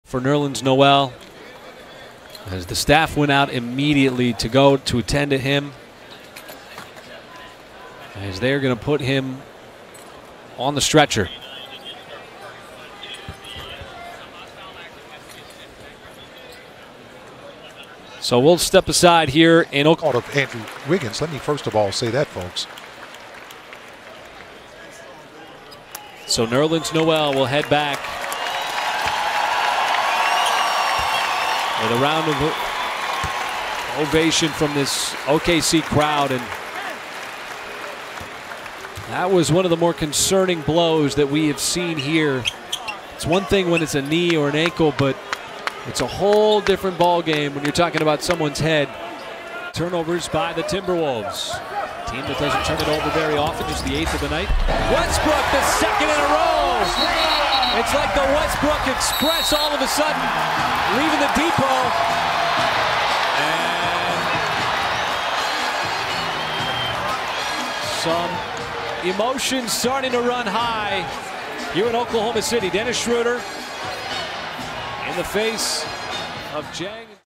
Thunder's Nerlens Noel Stretchered Off Court After Head Injury
Prayers up for Nerlens Noel. The Thunder big man took an elbow to the head and hit the deck hard. Dennis Schröder seemed to think the play was dirty, and got into it with Jeff Teague. Teague was ejected.